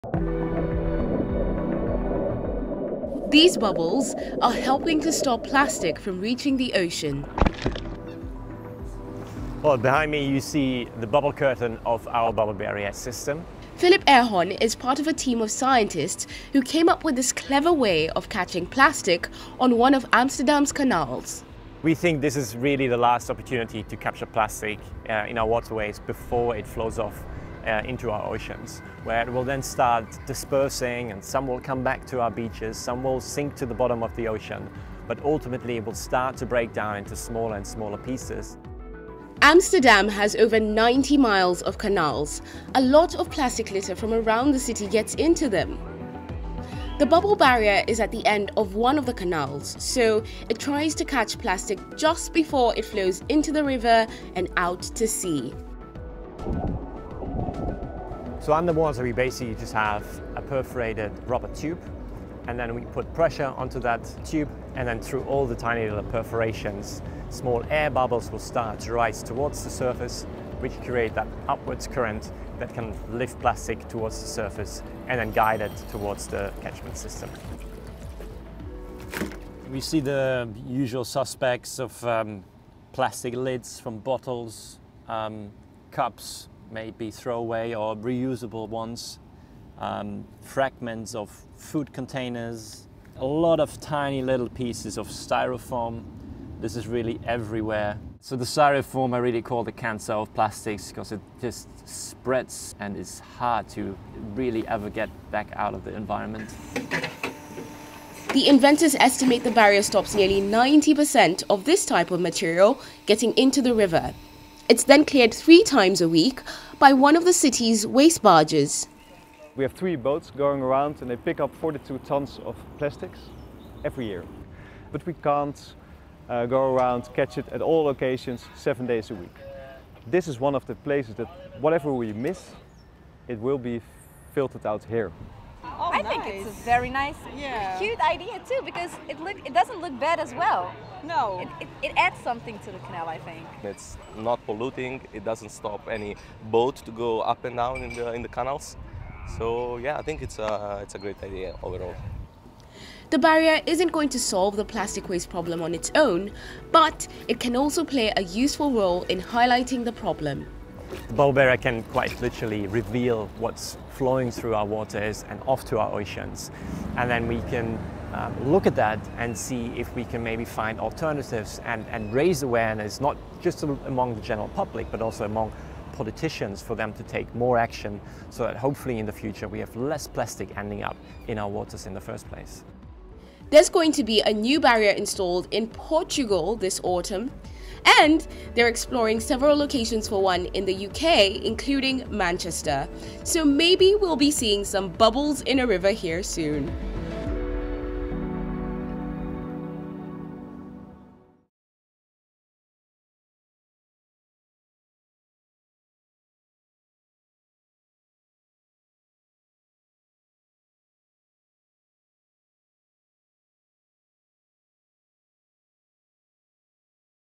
Amsterdam’s ‘bubble barrier’ fighting plastic pollution - BBC News
Dutch inventors are removing plastic from Amsterdam’s rivers and canals using a ‘net’ made from bubbles.

They claim the invention stops nearly 90% of plastic waste from passing downstream.

The BBC visited The Netherlands to see the barrier in action.